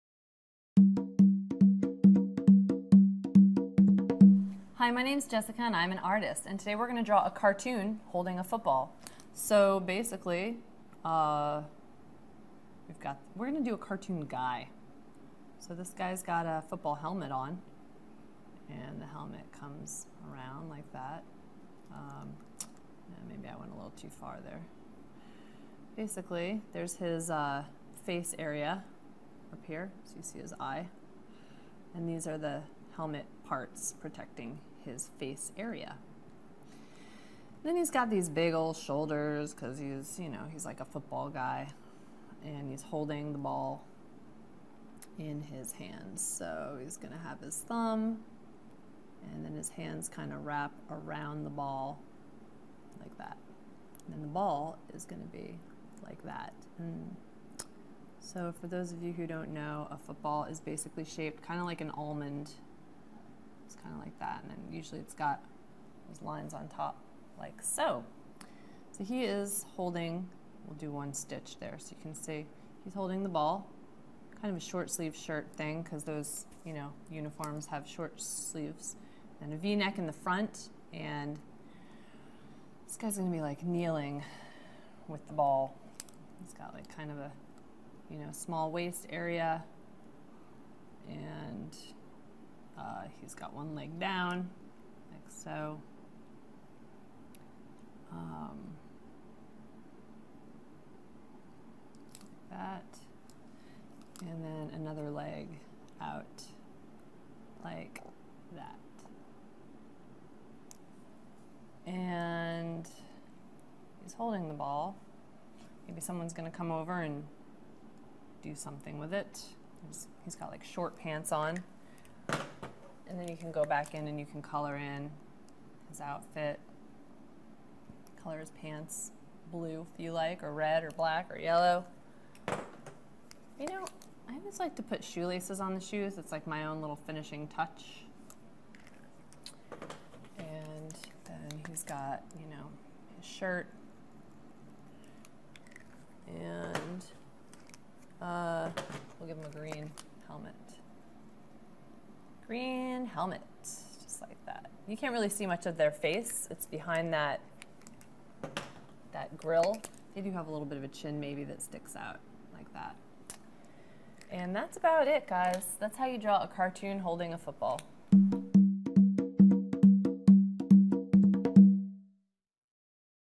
How to Draw a Cartoon Holding a Football
How to Draw a Cartoon Holding a Football. Part of the series: Drawing Help & Basics. Drawing a cartoon person holding a football requires you to exaggerate the features of both the character and the ball. Draw a cartoon holding a football with help from an artist in this free video clip.